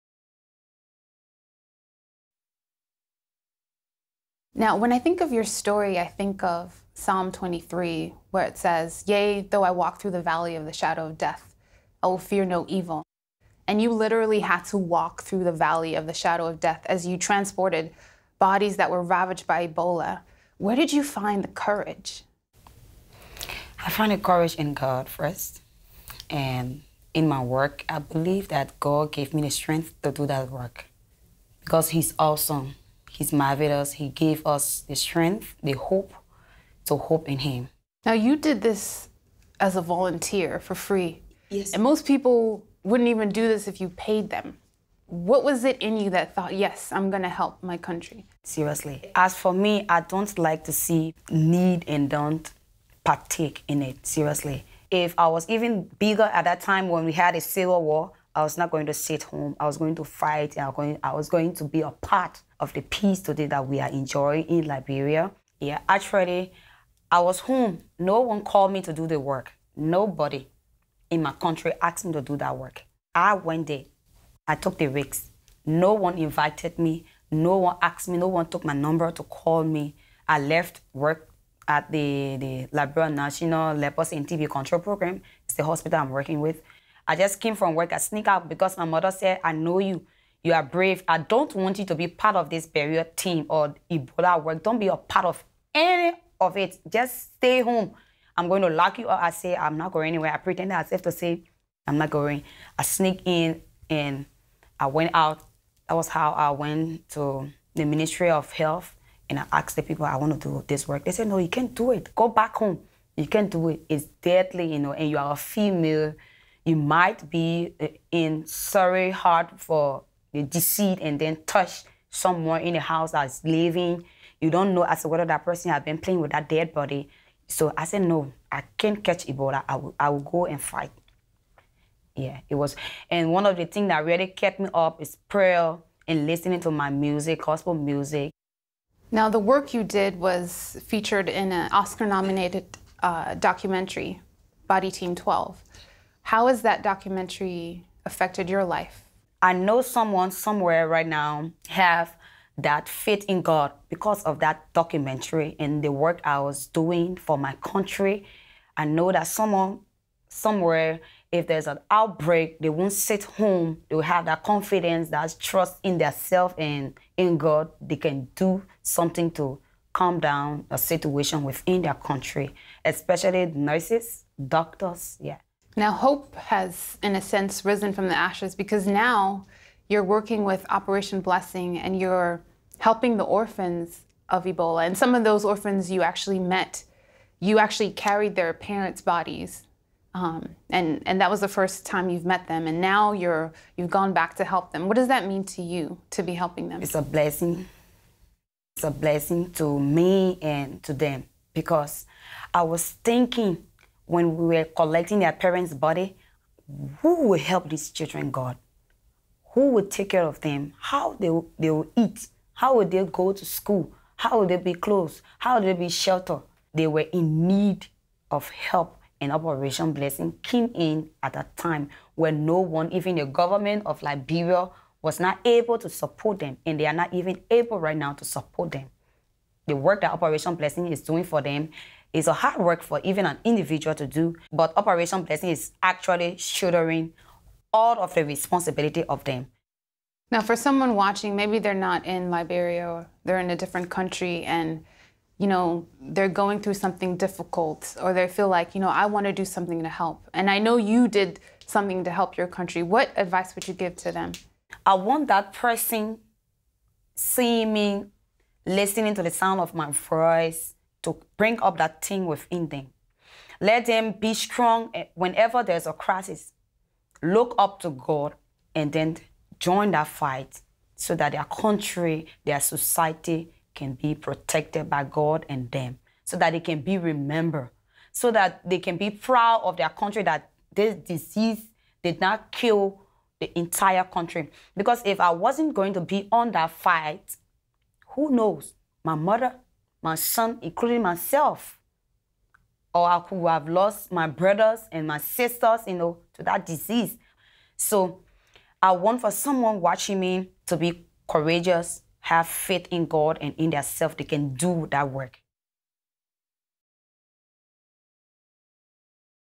She Collected Dead Bodies In The Height of The Ebola Outbreak | Garmai Sumo
She Collected Dead Bodies In The Height of The Ebola Outbreak and her story was nominated for an Oscar in the documentary "Body Team 12." She is the absolute definition of a hero. She is the quintessential she-ro